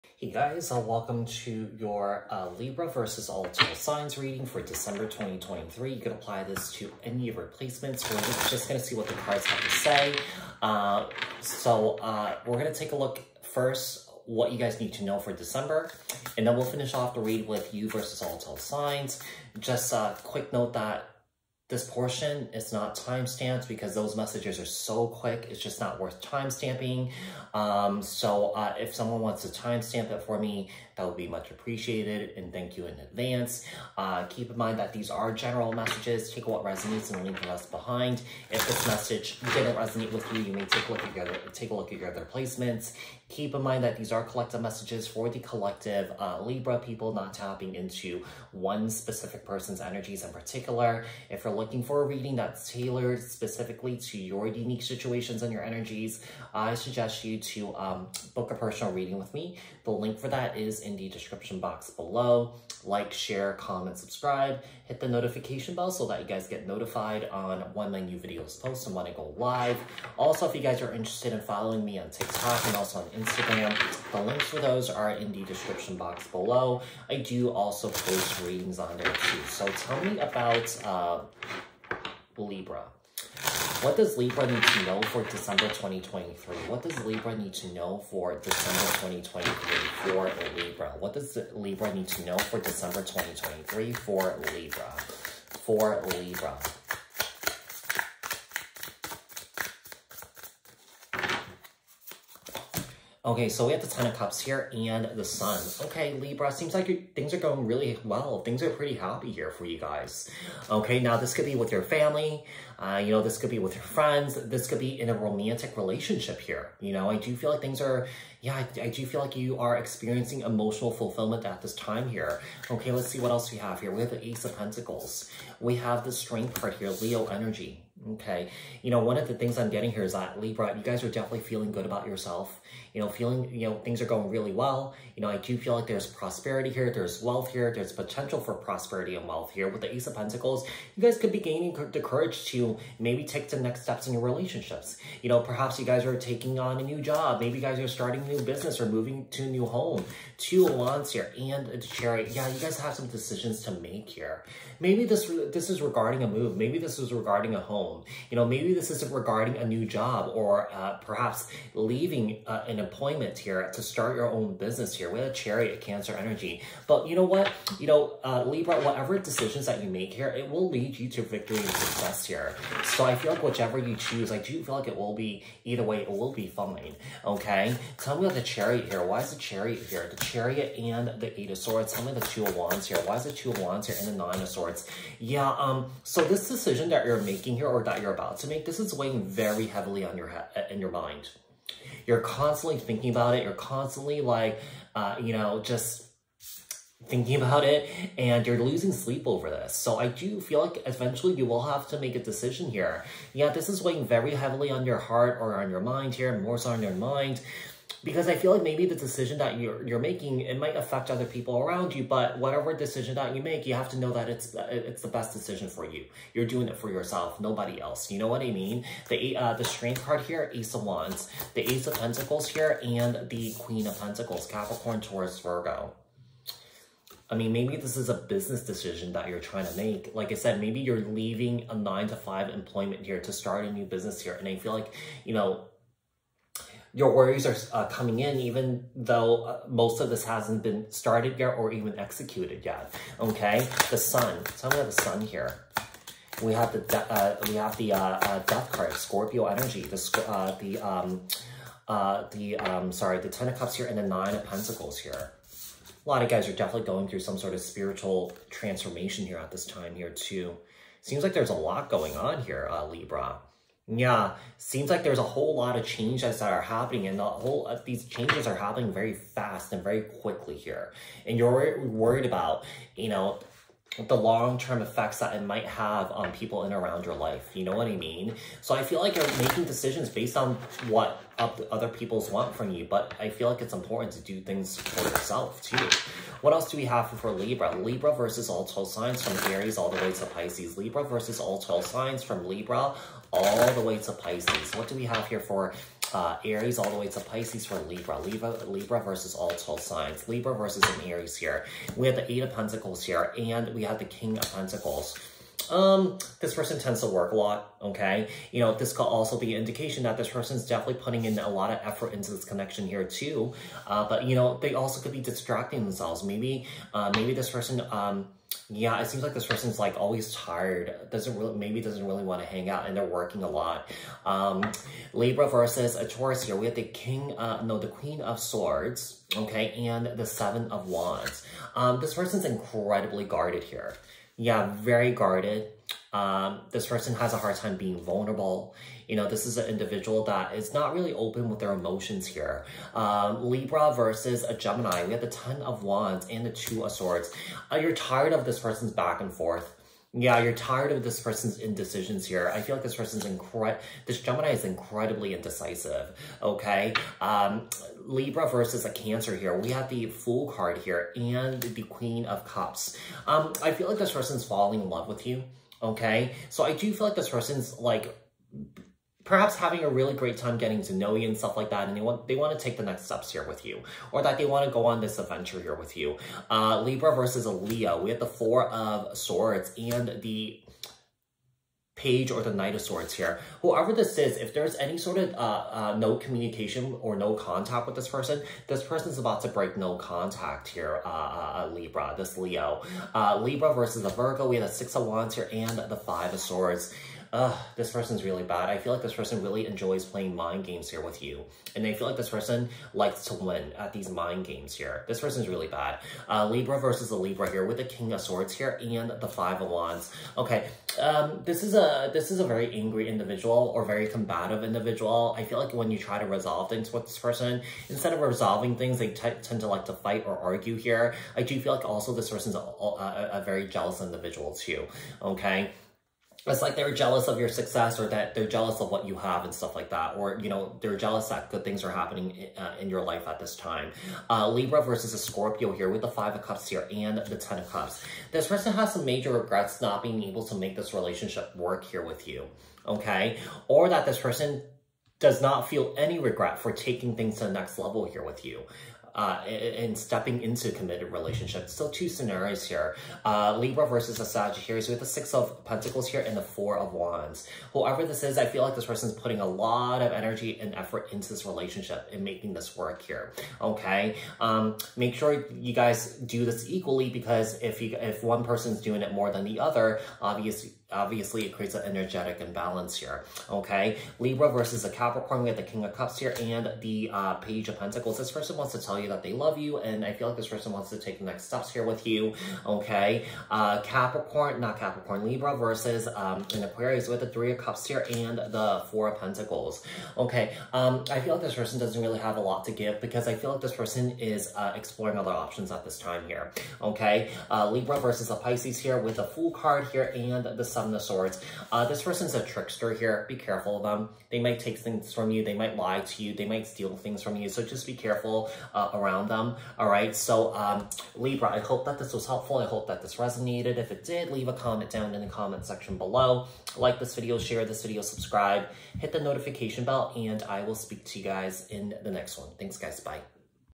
Libra ♎️ VS. All 12 Signs - What You Need To Know Right Now!!!!
This is an Libra VS All 12 Signs for December 2023. We are going to take a look and see what you need to know right now, and then we will finish off the read with you vs all 12 signs.

Decks used in this reading:
Magic Soul Tarot

duftes Deck